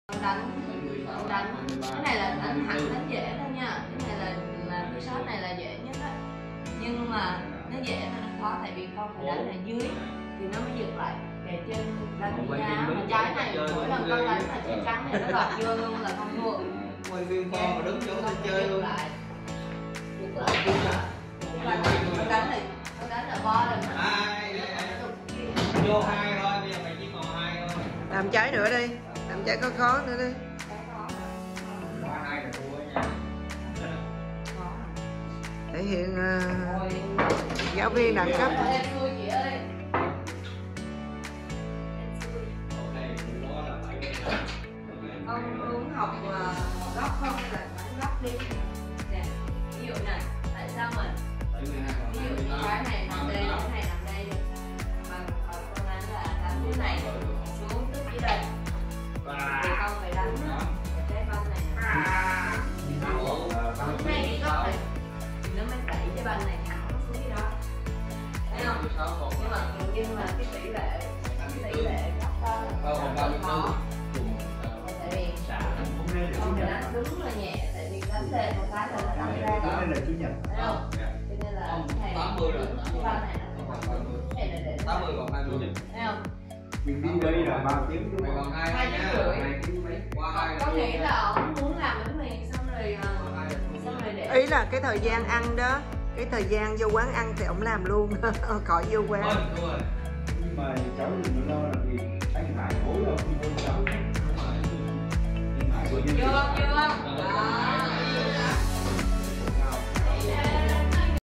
CA SĨ NHƯ LOAN HƯỚNG DẪN HOÀNG THIÊN NGA ĐÁNH BI-A Ở HOLLAND
CA SĨ NHƯ LOAN HƯỚNG DẪN JENNIFER THIÊN NGA ĐÁNH BI-A Ở HOLLAND

HOÀNG THIÊN NGA
QUÁN QUÂN CUỘC THI HÃY NGHE TÔI HÁT NHÍ 2020